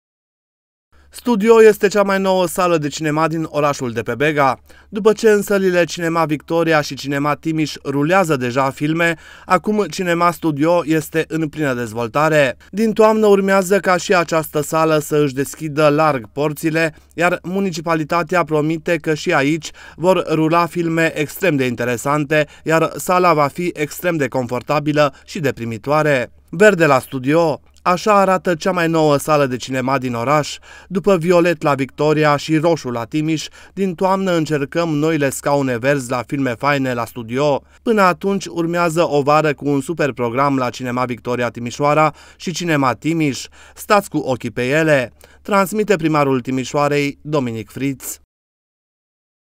STUDIO – CEA MAI NOUĂ SALĂ DE CINEMA DIN TIMIȘOARA
Studio este cea mai nouă sală de cinema din orașul de pe Bega. După ce în sălile Cinema Victoria și Cinema Timiș rulează deja filme, acum Cinema Studio este în plină dezvolare. Din toamnă urmează ca și această sală să își deschidă larg porțile, iar municipalitatea promite că și aici vor rula filme extrem de interesante, iar sala va fi extrem de confortabilă și de primitoare.